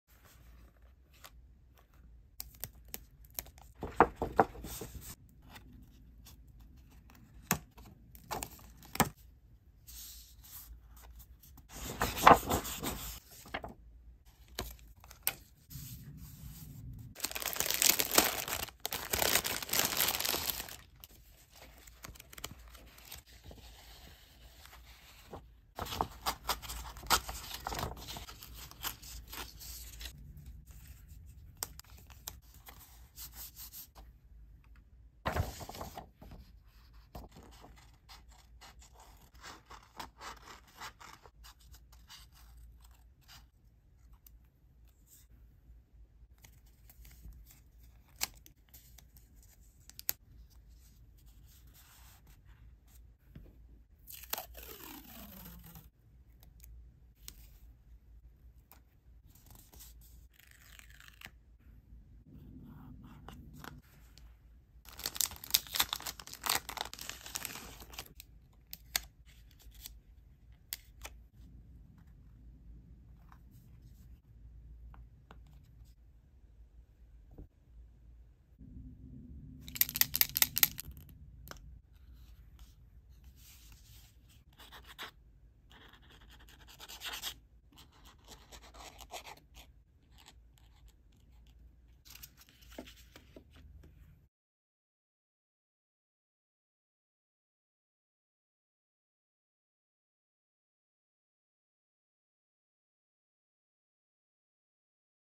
ASMR Journal With Me 🍂 Autumn Theme 🍁 Greeting Card Edition 🌾 Noemi LeNette Arts
3rd in the Greeting Card edition of the ASMR Journal With Me playlist.

We have a few blank Greeting Card sets available in the Poshmark shop.

I’m making these videos to show some ideas of possible card cover art.

On all social media @NoemiLeNetteArts 🦋